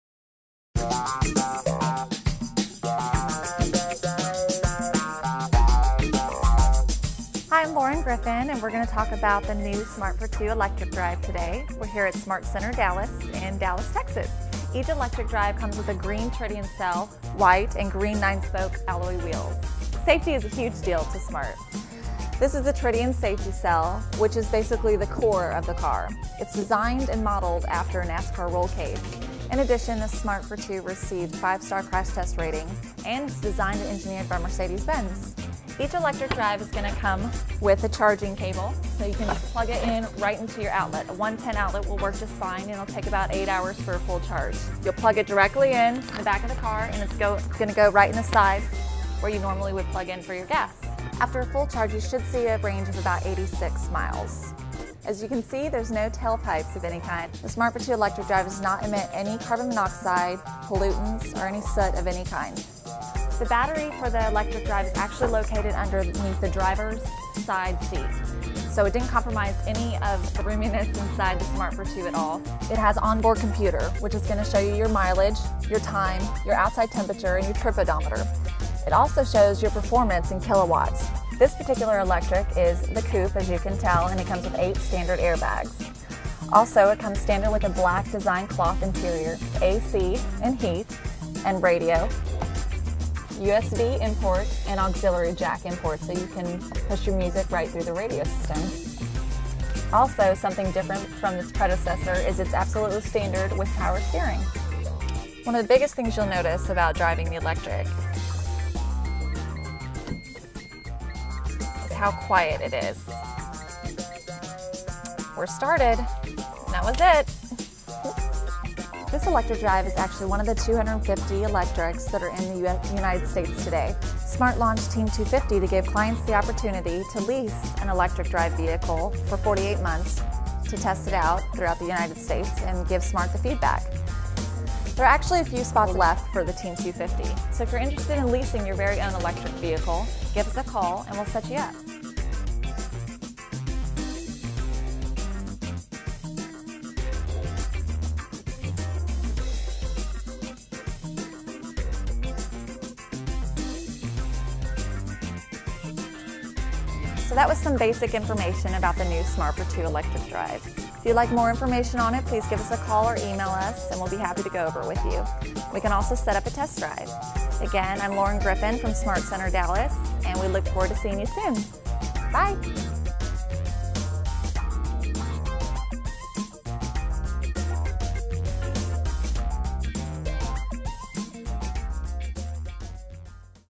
2011 smart electric drive
Lauren Griffin gives an overview of the smart electric drive. Only 250 will be available in the U.S. this year.